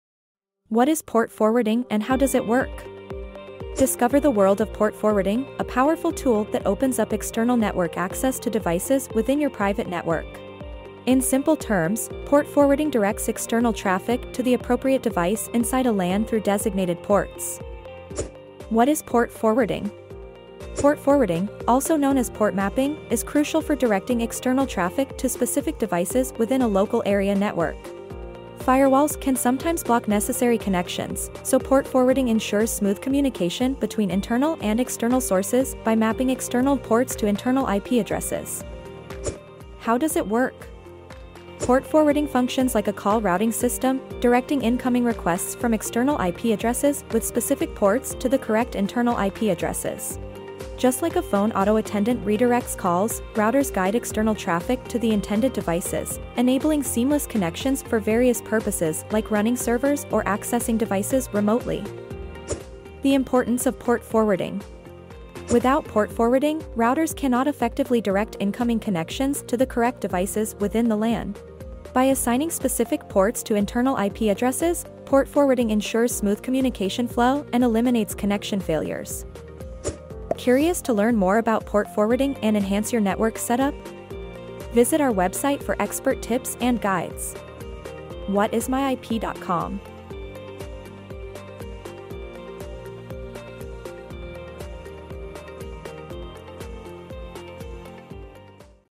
What Is Port Forwarding and How Does It Work?
🚀 Dive into the world of port forwarding with us! 📡👾

Unlock external network access to devices on your private network with this essential guide! We'll break down the concept of port forwarding, explain how it works, and highlight its importance for seamless connections in your LAN.

🔑 What is Port Forwarding?
   Learn how it directs external traffic to the right internal devices.

   Think of it as a call-routing system for your network, ensuring every request visits the right digital 'address'!

   Discover why port forwarding is a game-changer for avoiding connection hiccups and enhancing your network setup.